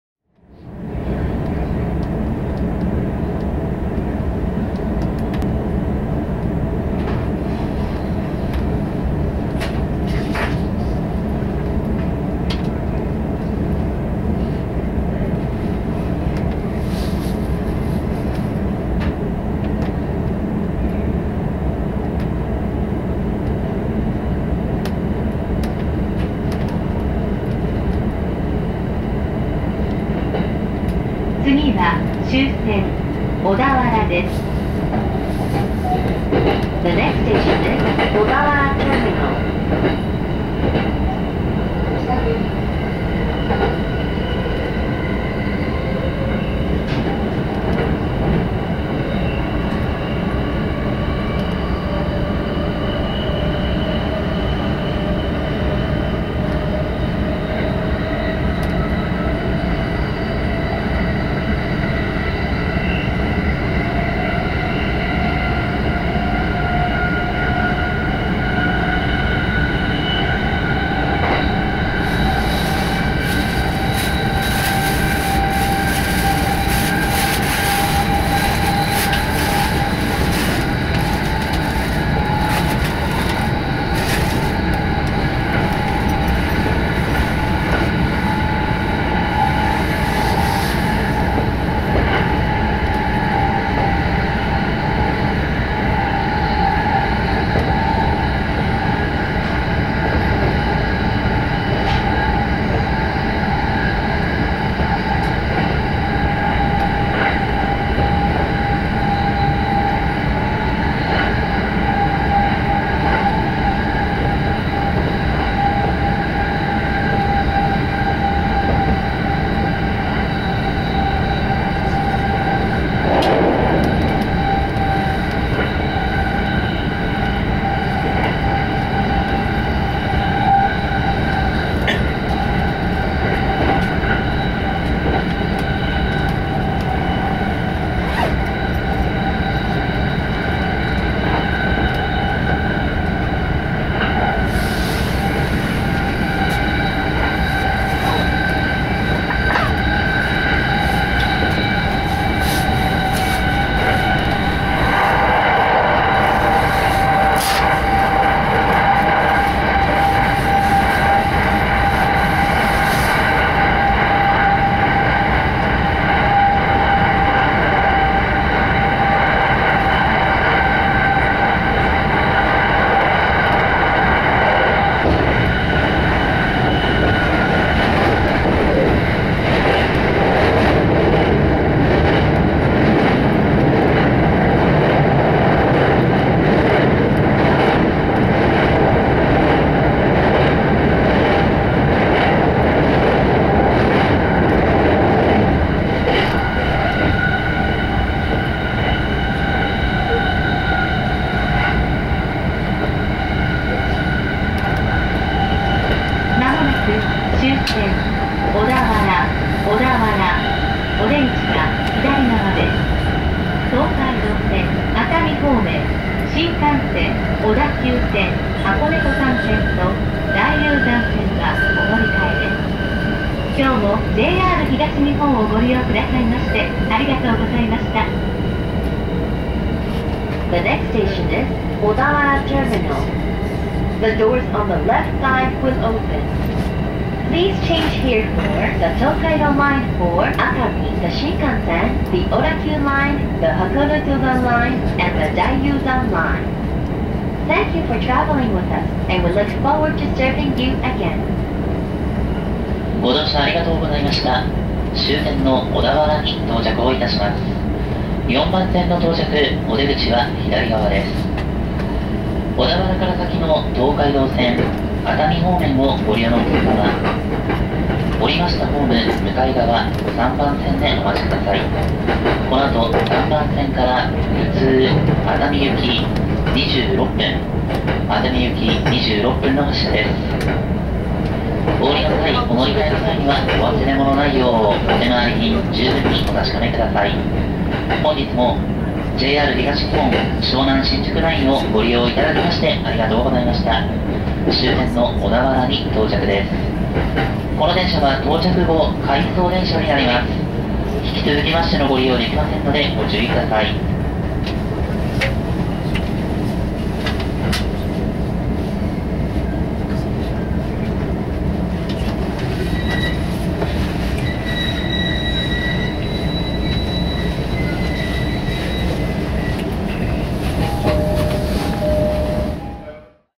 E231系 JR湘南新宿ライン 特別快速 国府津→小田原 側面展望
E231系 JR湘南新宿ライン 特別快速 小田原行き 5号車 グリーン車2階席 進行方向右側から撮影
国府津駅→小田原駅間の車窓です。